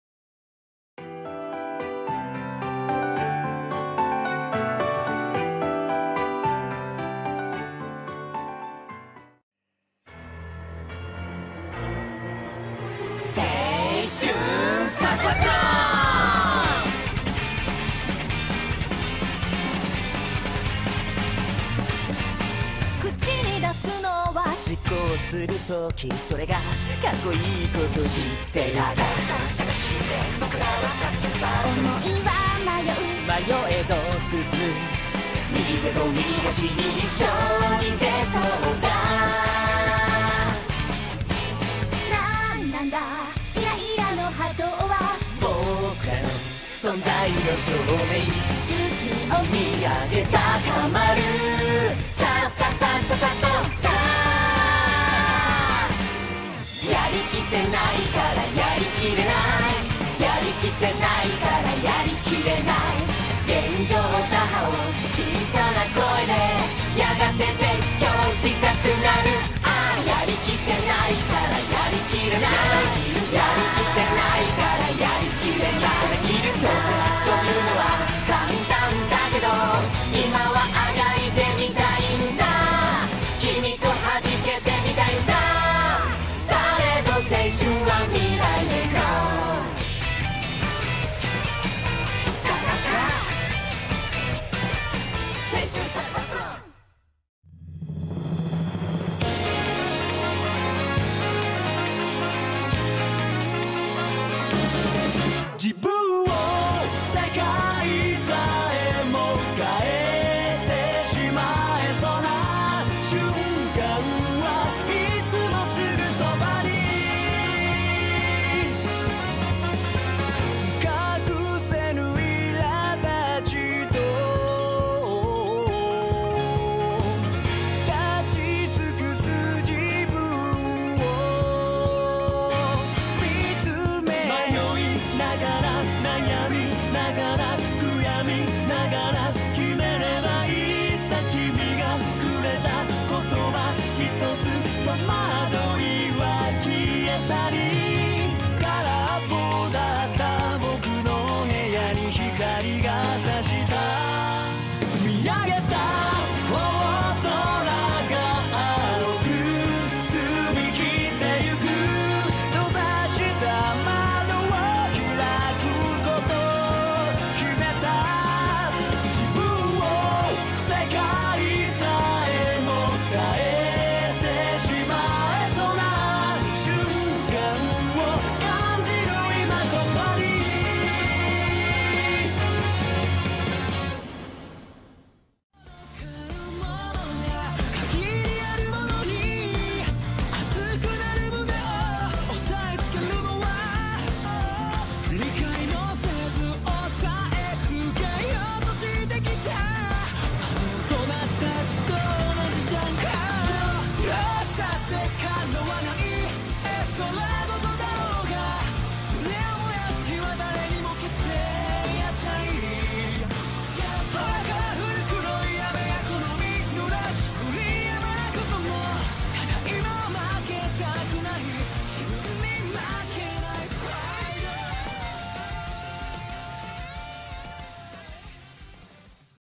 Seiyū Jun Fukuyama/Roles played in Anime
Hello, I know this is something new apart from the tops, but I think they are fun facts about the anime.I hope you like it, tell me if you want more. Thanks for waching ^-^